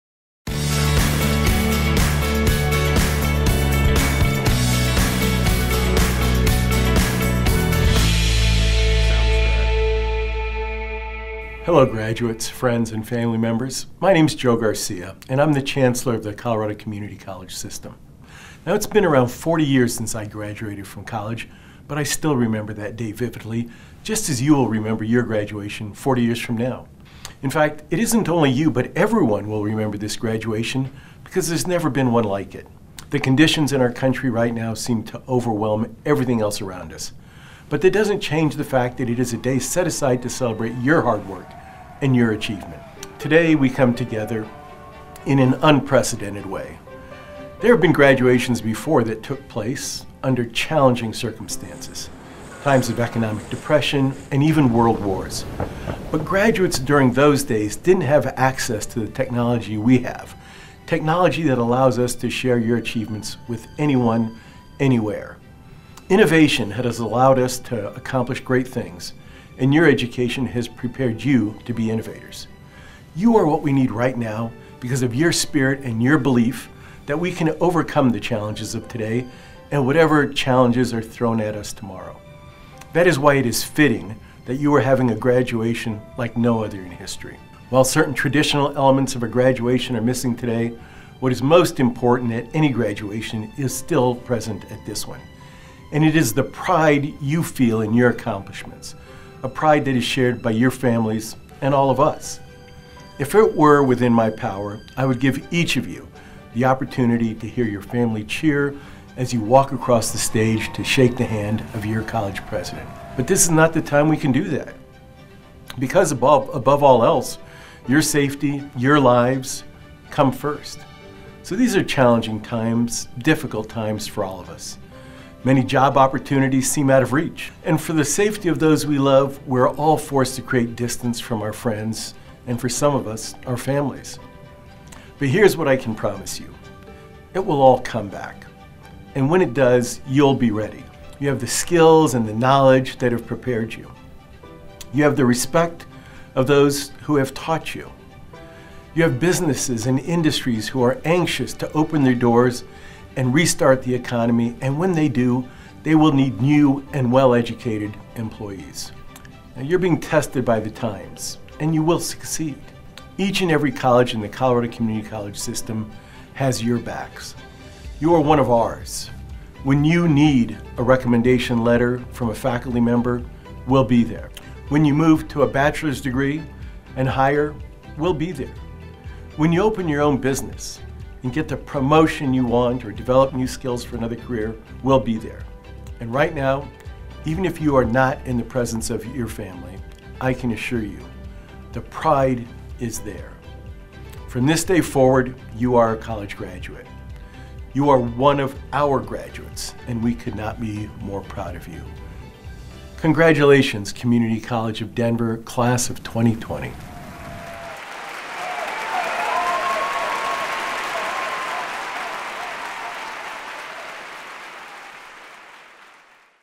Chancellor Joe Garcia Message to Graduates
Colorado Community College System Chancellor Joe Garcia offers message of inspiration and congratulations to Community College of Denver graduates.